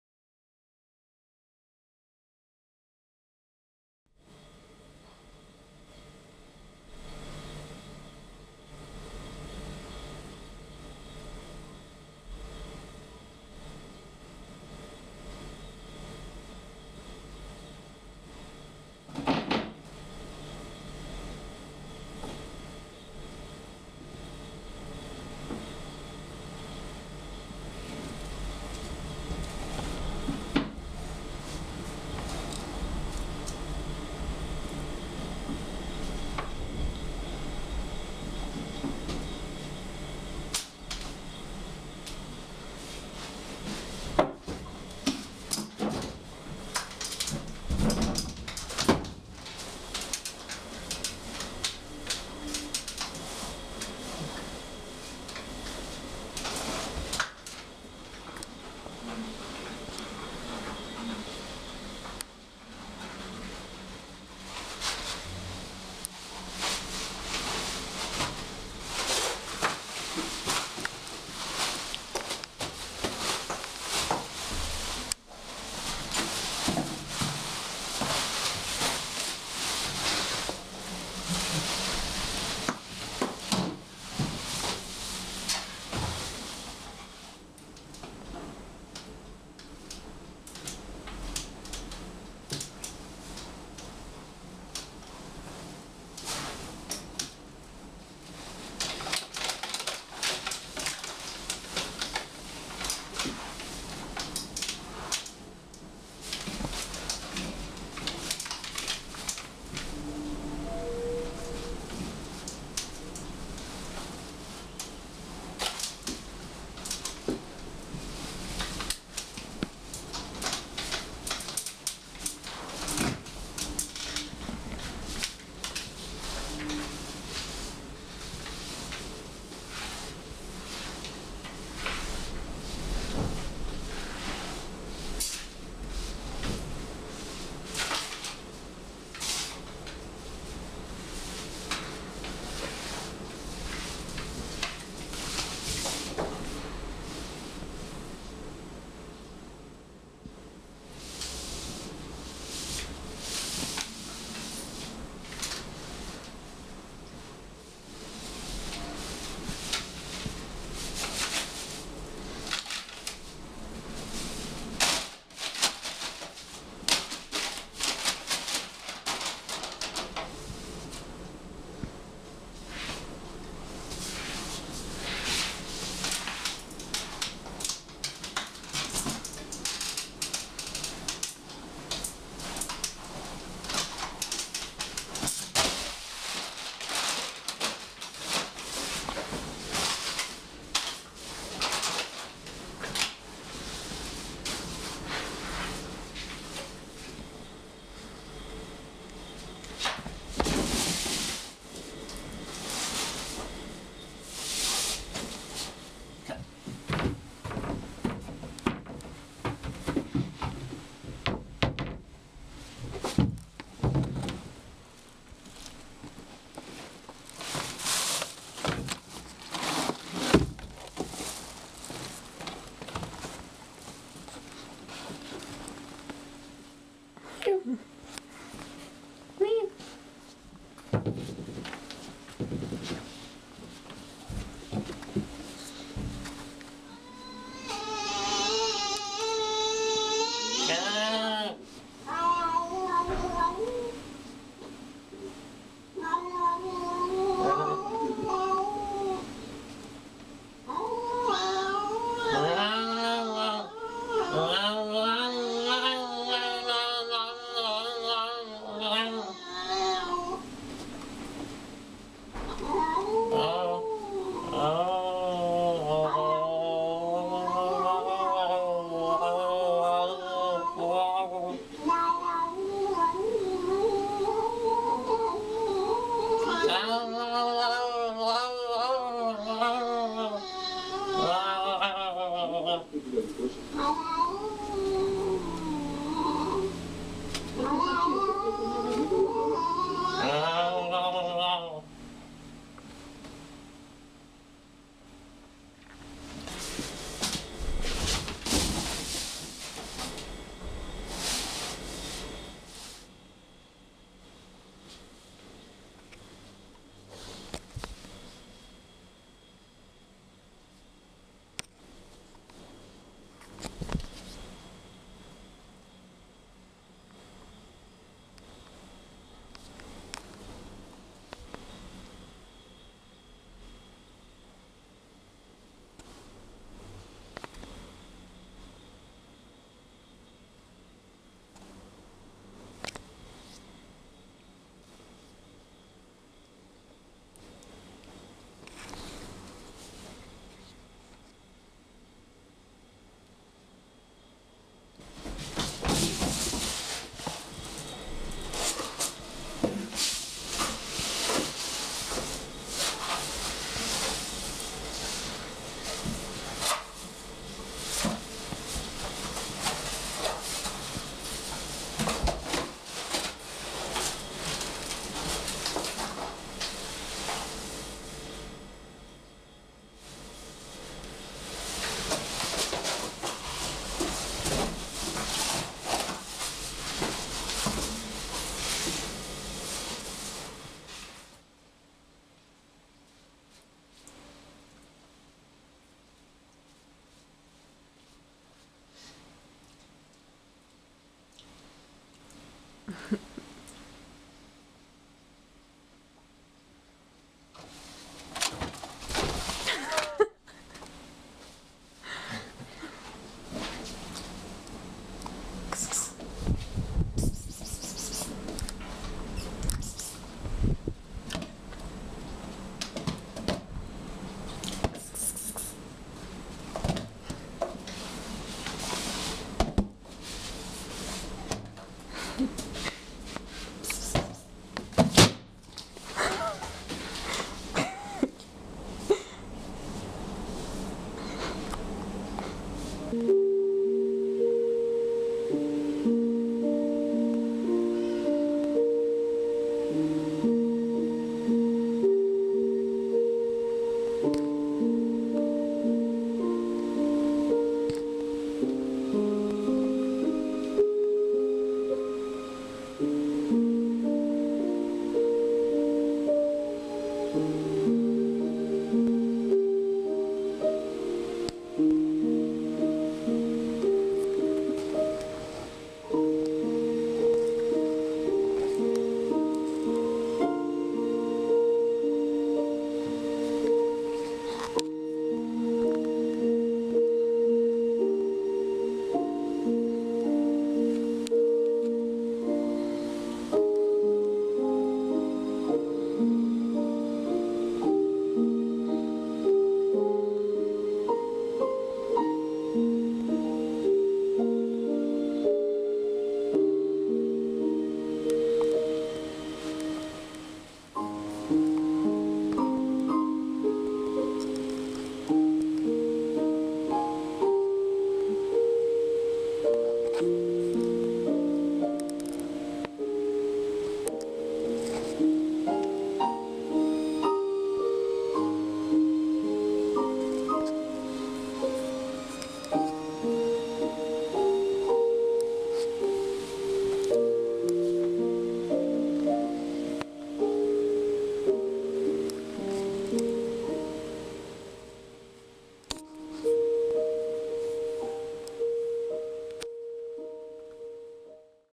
Cute Cats
This is my cat Tom.
He so furry!